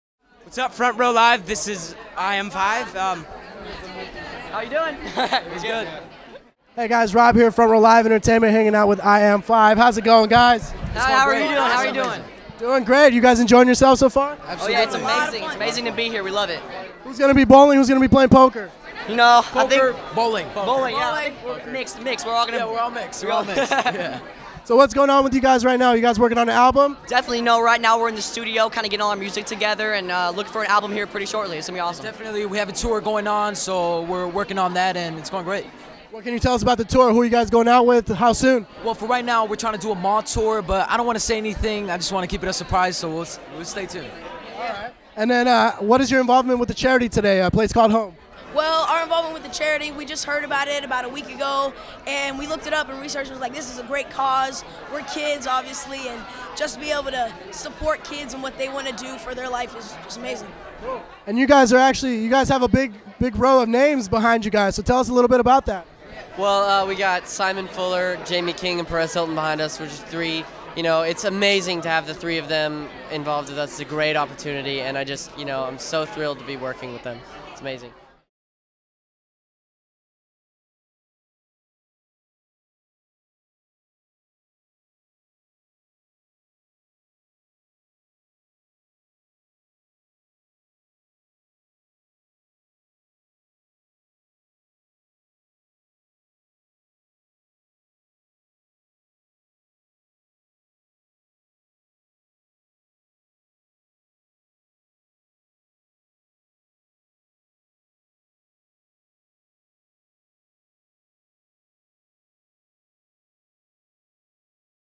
IM5 interview w/ Robert Herrera
Robert Herrera spoke with IM5 on the A Place Called Home red carpet event. The guys told us about their upcoming album, tour, how they formed and more...

Pinz Bowling
Studio City, California

Directed By: Frontrowliveent.com
Filmed By: Alberto Herrera
Photography By: P.rose
Interview By: Robert Herrera

Like my bracelet? Check Out BeadsMe